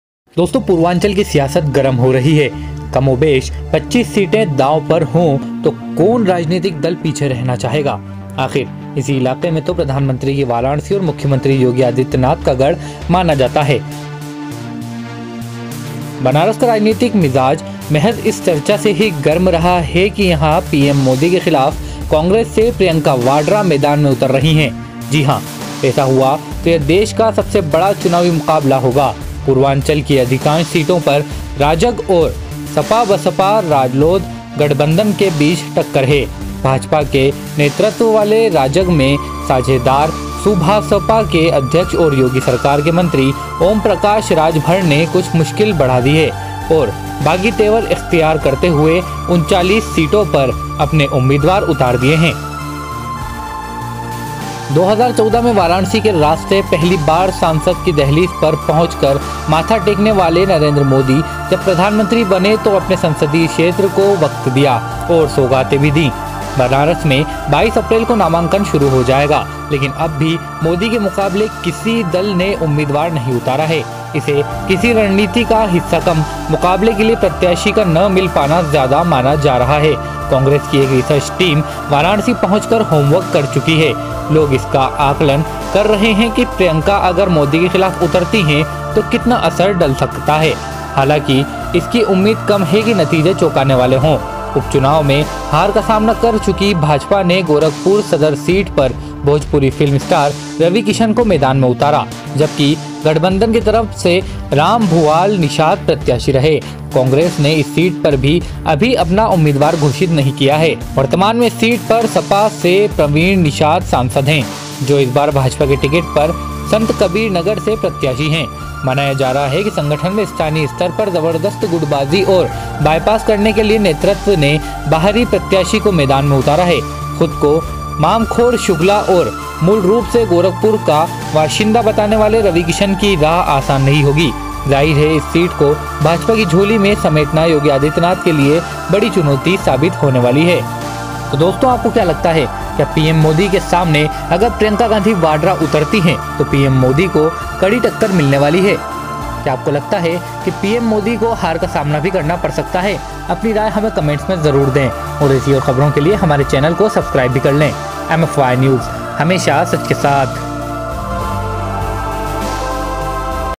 लोकसभा चुनाव - मोदी को भी होगी अब घबराहट,वाराणसी में कांग्रेस की तरफ से देंगी प्रियंका टक्कर
पूर्वांचल की सियासत गरम है। कमोबेश 25 सीटें दांव पर हों तो कौन राजनीतिक दल पीछे रहना चाहेगा। आखिर इसी इलाके में तो प्रधानमंत्री की वाराणसी और मुख्यमंत्री योगी आदित्यनाथ का गढ़ मानी जाने वाली गोरखपुर की सीटें आती हैं।